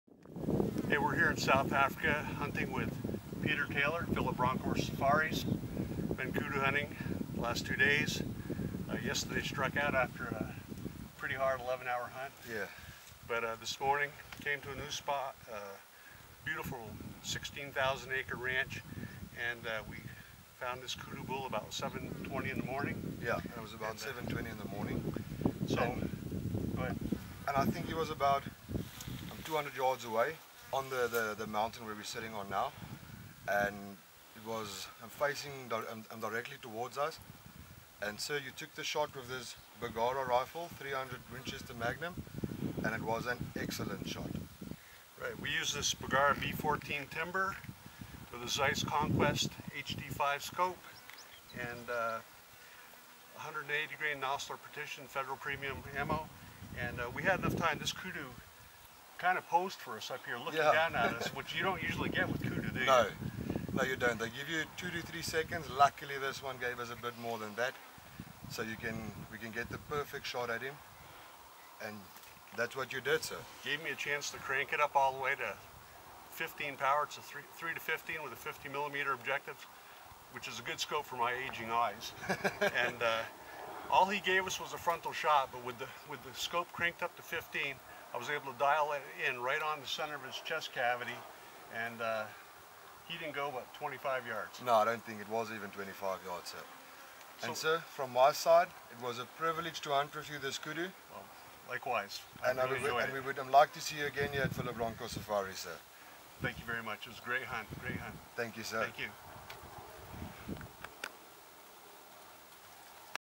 South Africa Kudu hunt with Bergara B14 Timber
Ken Perrotte and Professional Hunter Pieter Taylor of Phillip Bronkhorst Safaris discuss a challenging shot on a greater southern kudu using a new Bergara B14 Timber rifle chambered in .300 Win. Mag.  The kudu's spiral horns taped out at 52.75 inches.